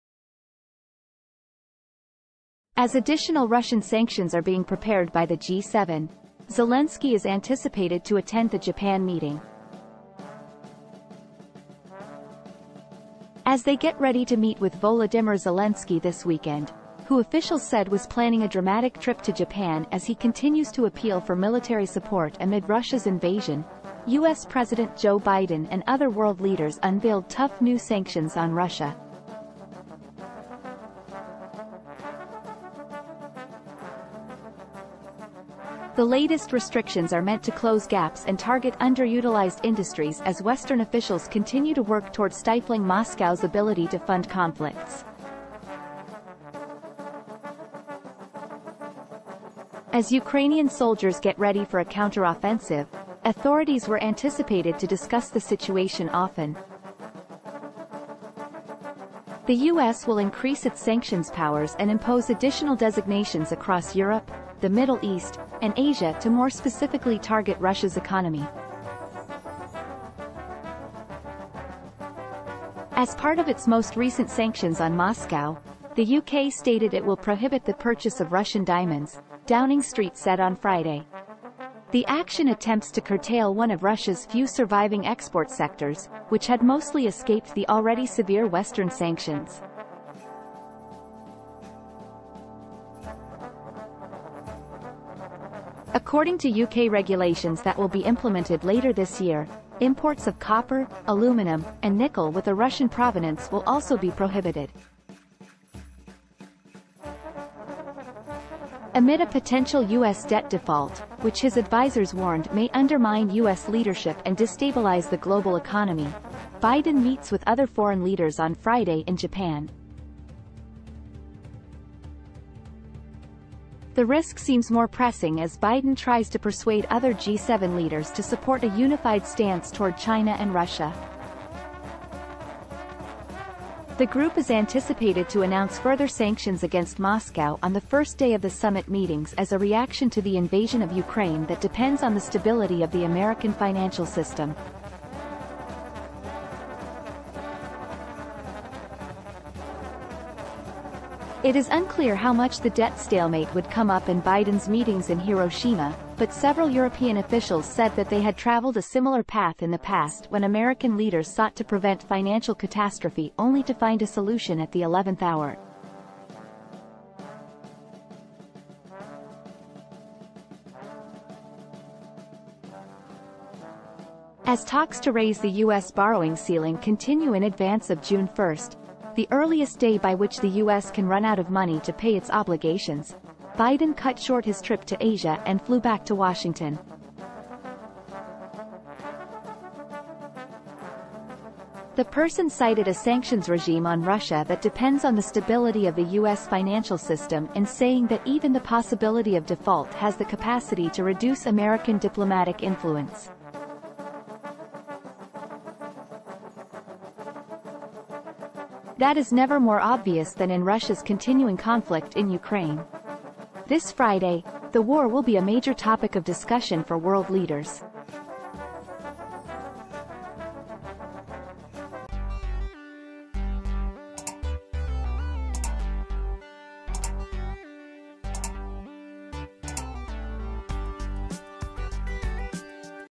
As Additional Russian Sanctions are Being Prepared By The G7 - VAXA Post
As Additional Russian Sanctions are Being Prepared By The G7, Zelensky is Anticipated to Attend The Japan Meeting